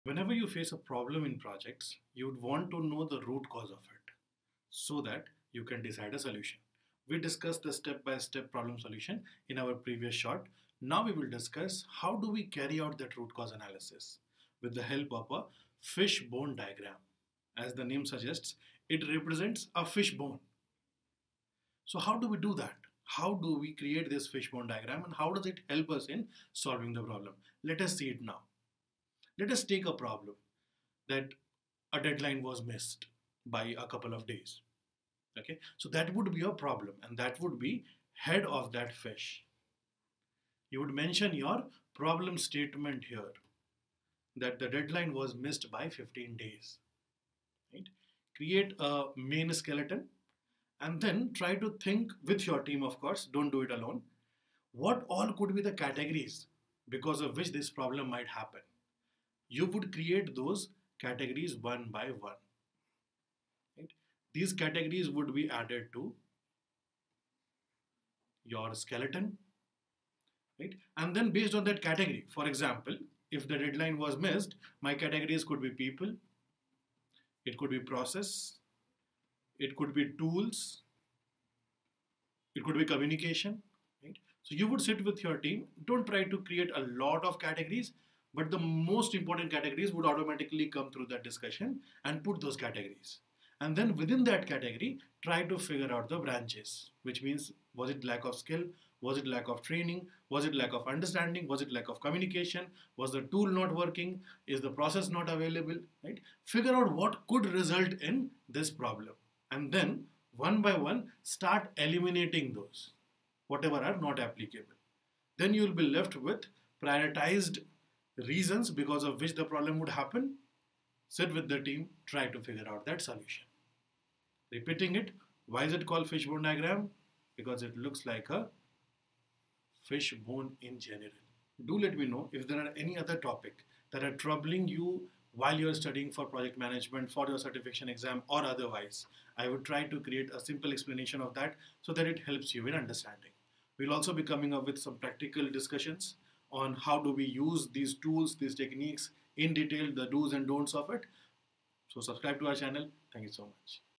Fish-Bone Diagram Explained in 150 Seconds 🐟 | Root Cause Analysis Made Easy!
Ever wondered how to identify the root cause of a problem?
In this quick and powerful video, we break down the Fishbone Diagram (also known as the Ishikawa Diagram) — a key tool in quality management and project problem-solving.

You'll learn:
✅ What the Fishbone Diagram is
✅ How to use it step-by-step
✅ Real-world applications for teams and managers
✅ Why it’s essential for PMP® & Six Sigma professionals

Perfect for students, professionals, and anyone who wants to solve problems smarter, not harder.

👉 Follow for more PM tools in under a minute!